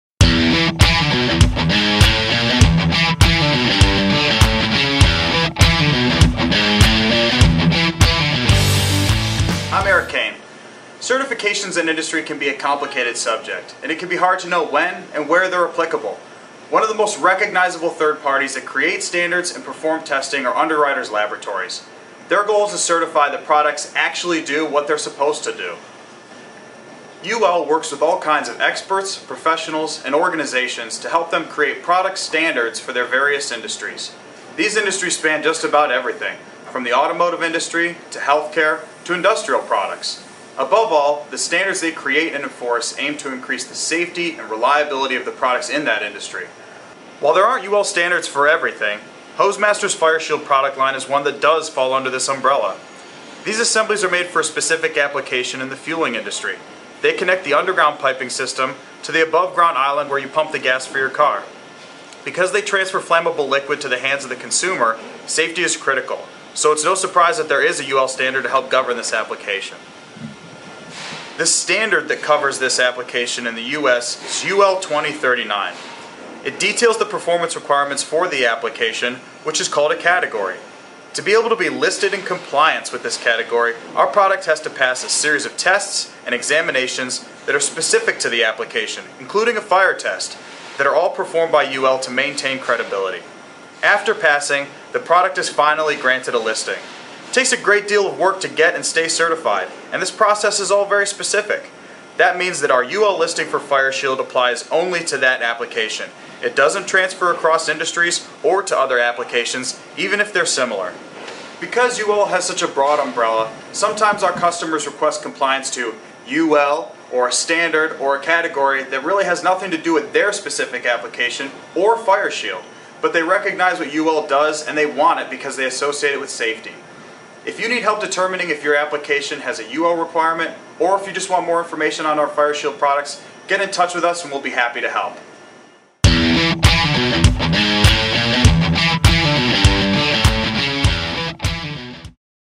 Understanding UL
Why would a customer need a UL-approved product, and when is the approval necessary?

► Certifications can be tricky to understand! When do we need which ones?!

► UL (Underwriter's Laboratories) is one of the major 3rd-party testing agencies.  UL works to create and enforce standards by working with those industries to ensure that products actually do what they say they're doing!

► Hose Master's FireShield product line is one that DOES fall under the UL umbrella!

► UL standards and listings are very specific and in order to be in compliance, it requires a lot of work to get tested and certified! This means that the UL certification is only good for a specific range of applications!